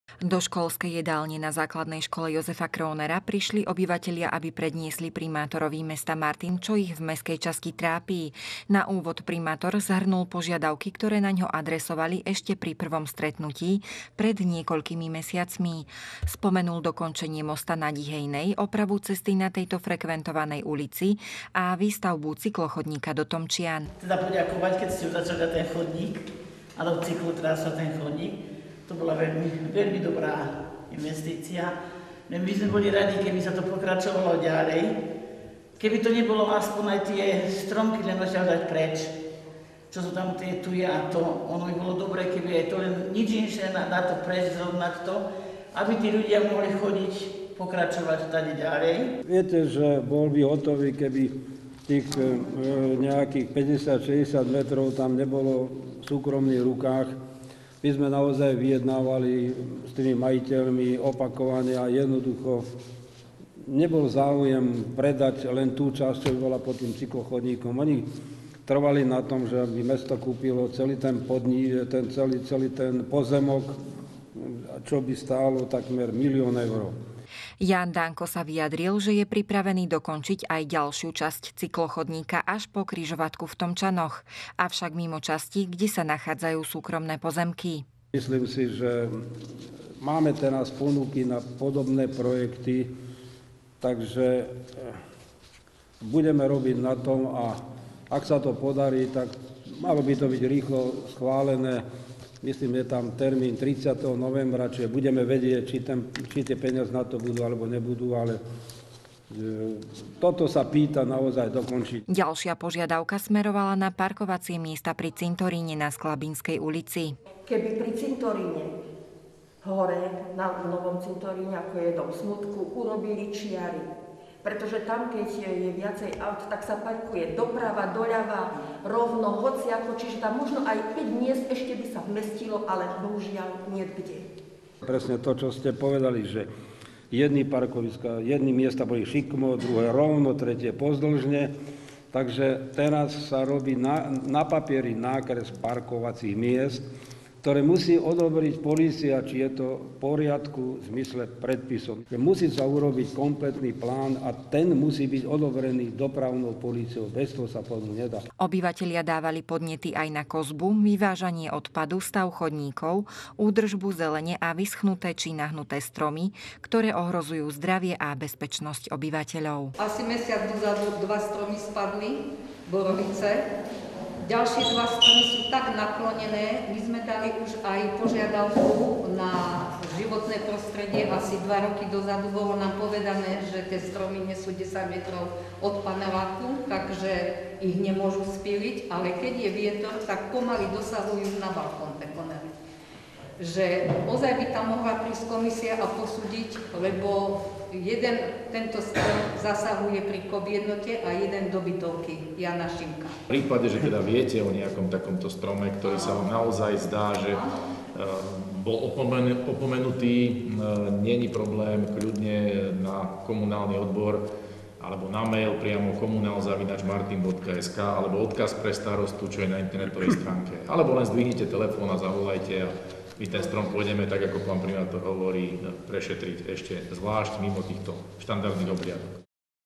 Deň s MČ Ľadoveň- Jahodníky-Tomčany
Obyvatelia Ľadovne Tomčian a Jahodník predniesli na stretnutí s primátorom svoje požiadavky. Diskutovalo sa o tom, že časť Jahodníky je v rámci tejto mestskej časti na okraji záujmu. Požadovalo sa vyznačenie čiar pre parkovacie miesta pri martinskom cintoríne. Ľudí trápi aj nedokončený cyklochodník do Tomčian, stav chodníkov aj údržba zelene.